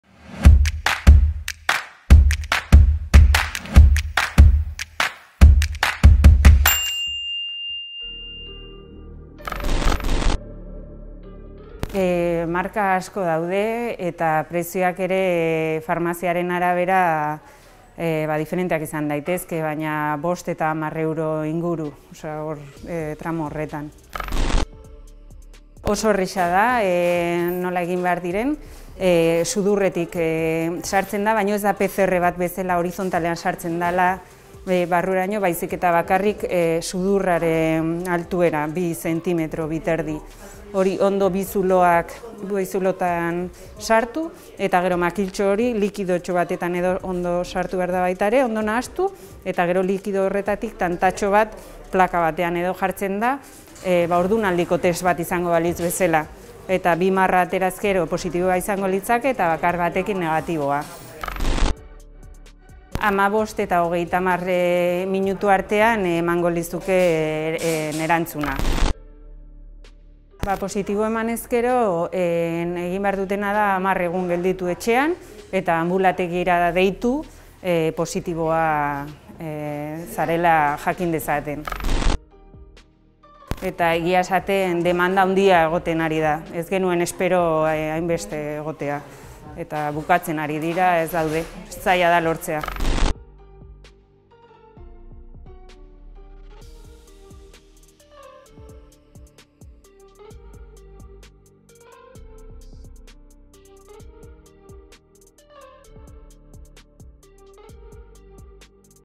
Antigeno testak
| Antigeno testen inguruan elkarrizketatu dugu Lierni Mendiaraz farmazeutika. Ondorengo galderei erantzuten die

Kapituluak:

00:00 Antigeno testak
00:11 Antigeno testen eskaintza
00:24 Nola egiten da antigeno testa?
01:01 Noiz jasotzen da antigeno testaren emaitza?
01:09 Antigeno testean positibo emanez gero?
01:22 Antigeno testen eskaria